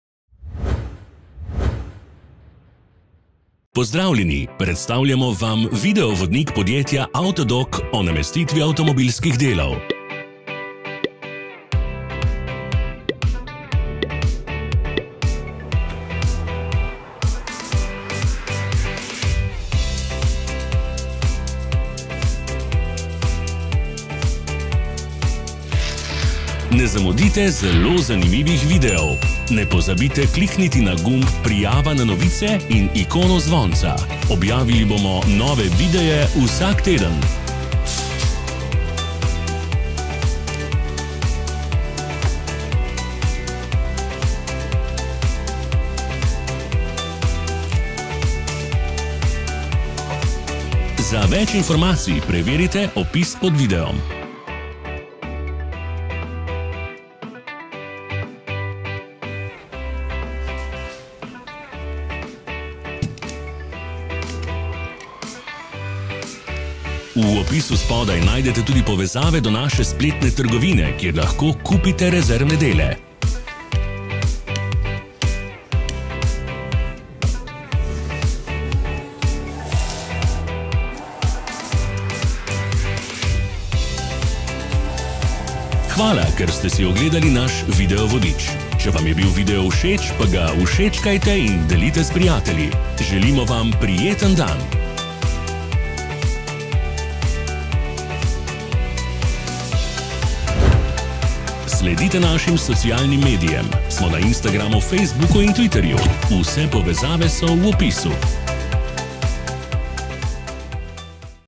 Kako zamenjati brisalci na VW PASSAT B6 (3C5) [VODIČ AUTODOC]
Kako zamenjati brisalci na VW PASSAT B6 (3C5) 2.0 Kombi 2005–2010 [VODIČ AUTODOC]

Brisalci na AUDI Q3 (8UB, 8UG) (06.2011–...) / SEAT Alhambra II (710, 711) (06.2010–...) / VW CC –...) / VW Passat B7 Variant –12.2014) / VW Tiguan I (5N) (09.2007–...) avtomobilih je praviloma potrebno zamenjati po enakem postopku.

🔝 Top znamke, ki proizvajajo brisalci: BOSCH; VALEO; HELLA; SWF; CHAMPION.

Vsi video in pdf vodiči za VW PASSAT B6 menjavo delov: prenesite in poglejte v AUTODOC CLUB